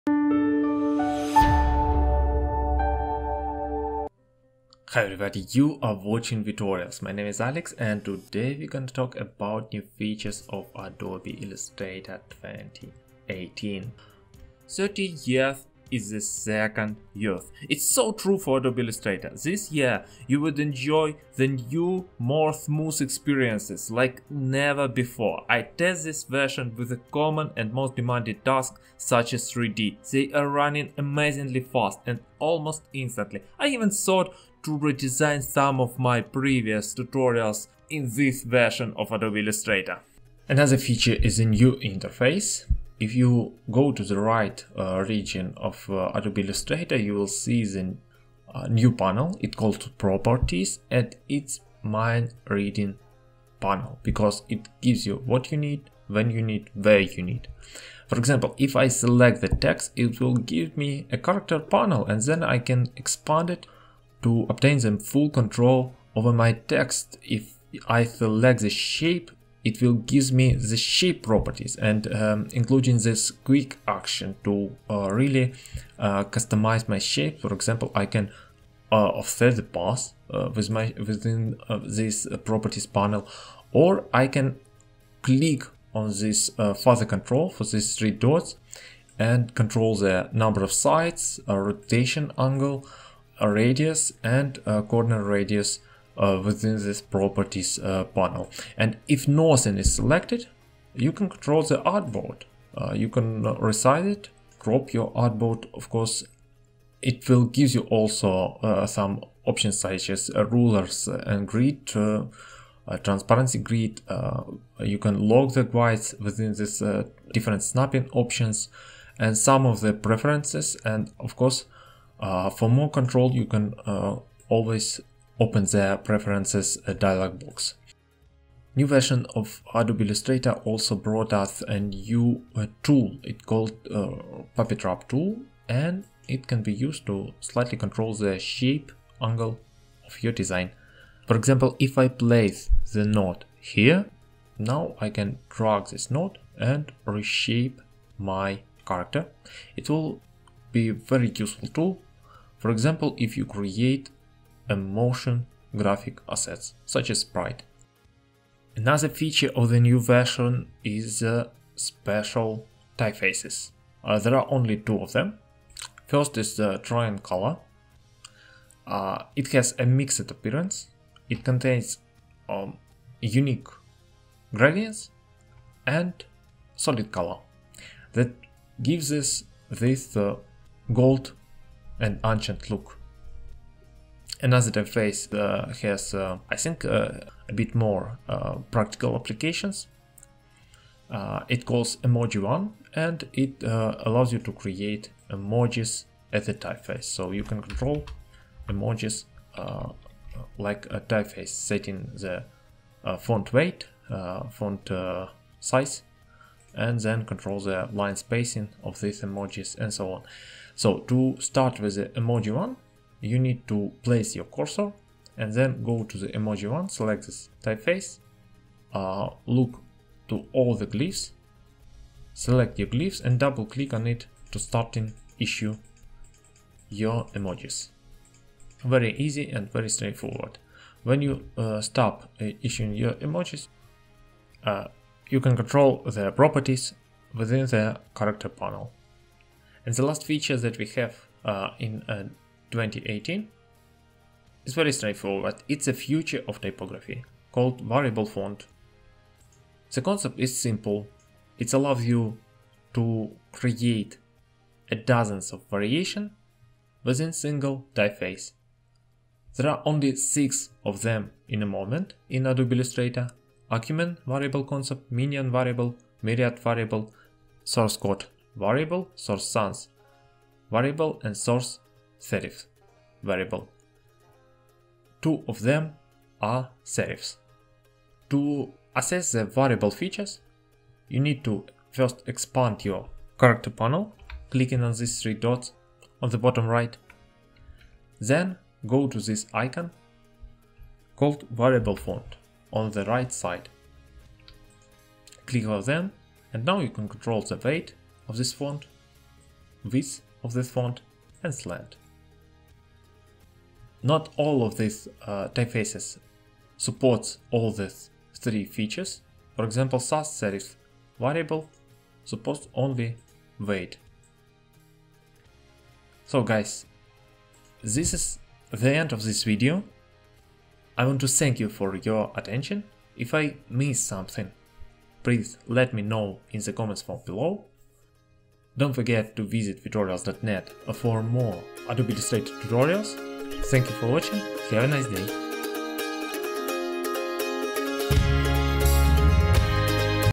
Adobe Illustrator CC 2018. Top 5 new features
Hi, folks. In today's video I am going to introduce top 5 new features of Adobe Illustrator CC 2018.
First and far most is a performance improvement. Adobe Illustrator is flying like a rocket now! I Tested it with some of most demanding tasks, including 3d and was amazed by productivity.
Most of rendering were so smooth, that I even thought about redesigning of my previous blend tutorials.

List of all features highlighted in the video:
1. Performance:
2. Properties panel:
3. Puppet warp tool:
4. Special typefaces ( Trajan Color and Emoji One ):
5. Variable fonts: